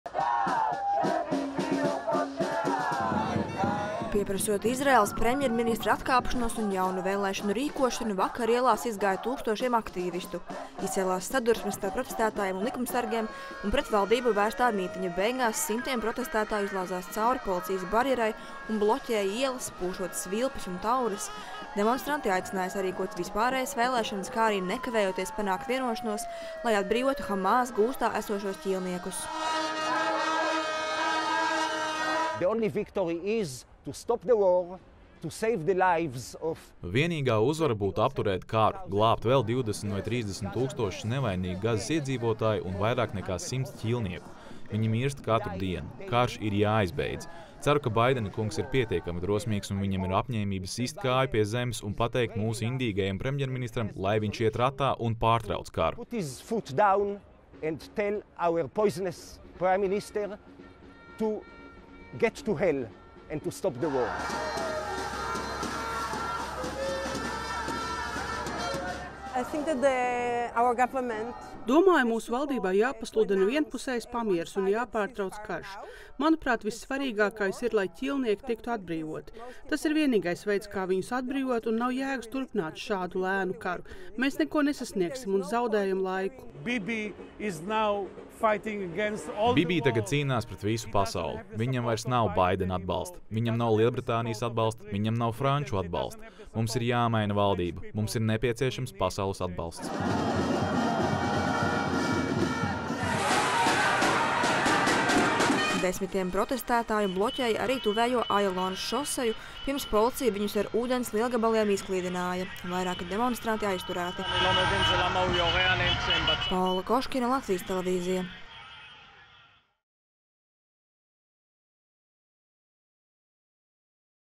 Protestētāji Telavivā pieprasa B. Netanjahu atkāpšanos
Nemierīgs sestdienas vakars bijis Telavivā. Tur ielās izgāja tūkstošiem demonstrantu, izrādot neapmierinātību par to, kā Izraēlas premjerministrs Benjamins Netanjahu risina jautājumu par “Hamās” saņemto ķīlnieku atbrīvošanu un karu kopumā.